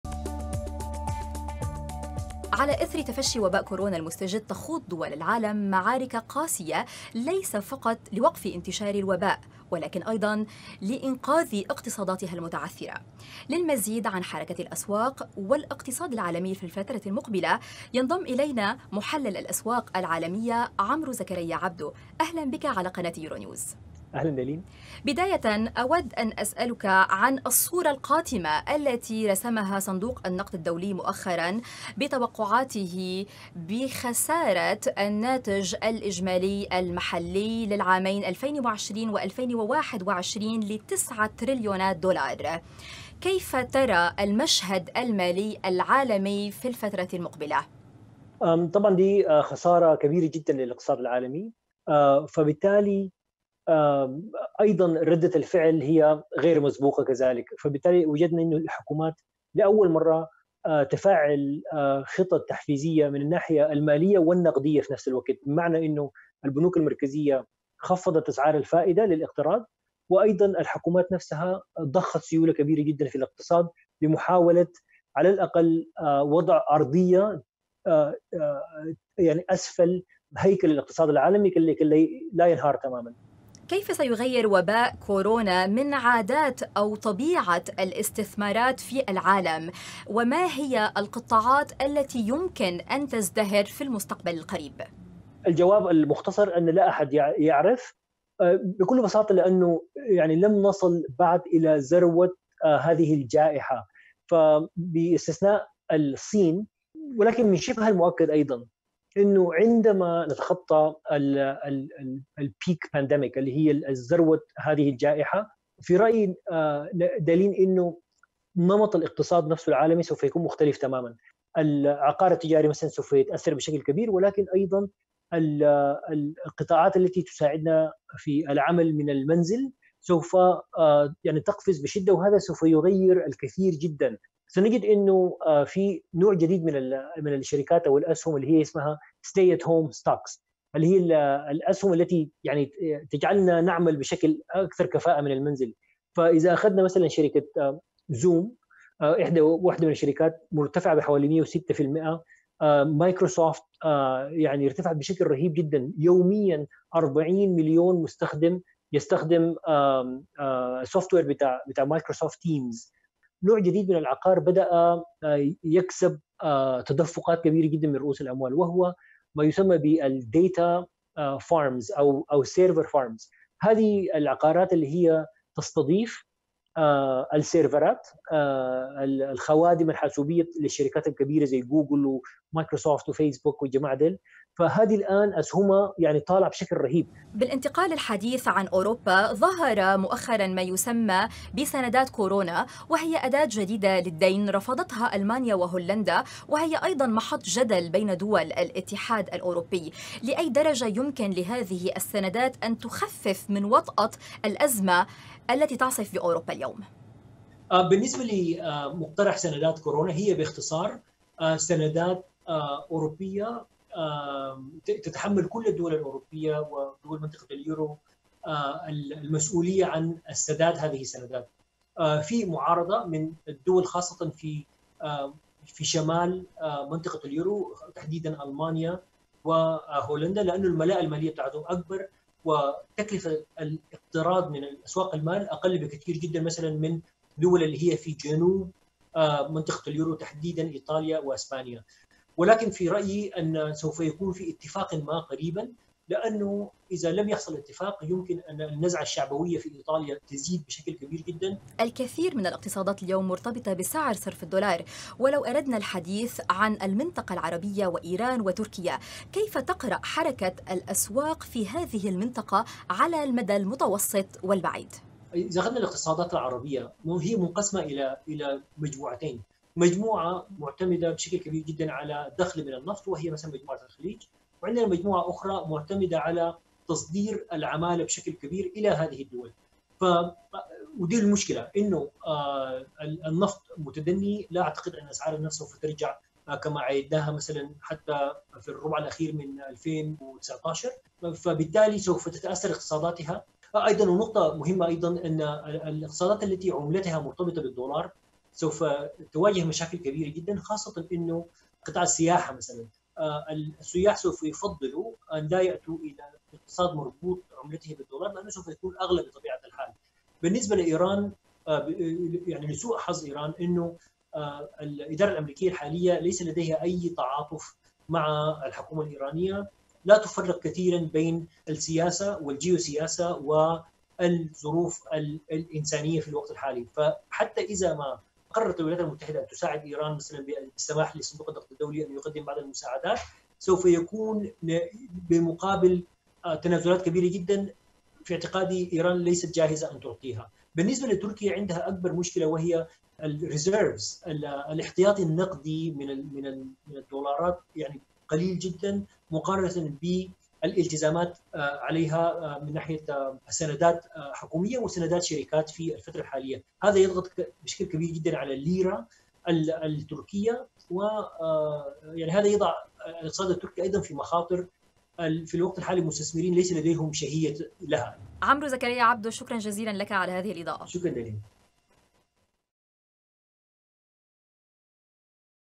خبير اقتصادي ليورونيوز: معاناة كبيرة للاقتصادات العربية المصدرة للنفط والعمالة جراء كورونا
يورونيوز : القناة الأكثر مشاهدة في أوروبا